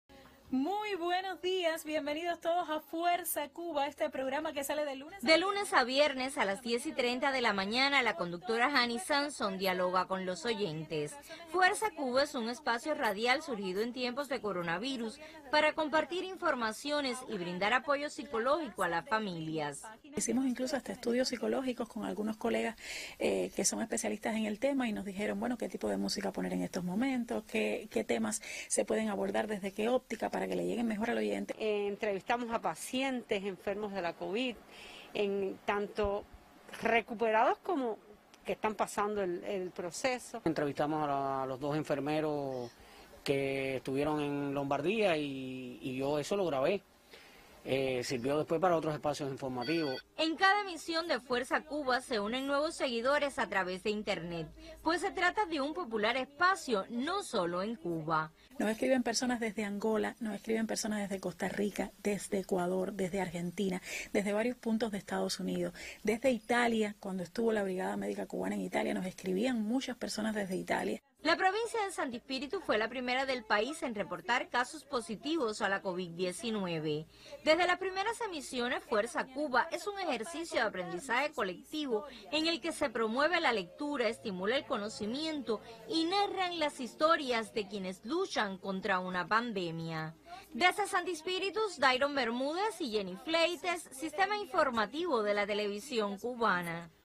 Fuerza Cuba: uno de los espacios más seguidos de la radio en Sancti Spíritus
El programa radial FuerzaCuba de la Emisora RadioSanctiSpíritus se ha convertido en uno de los espacios más seguidos en ese territorio, en otras provincias y en decenas de países a través de internet. Durante varios meses han acompañado a los oyentes en los difíciles días de una pandemia.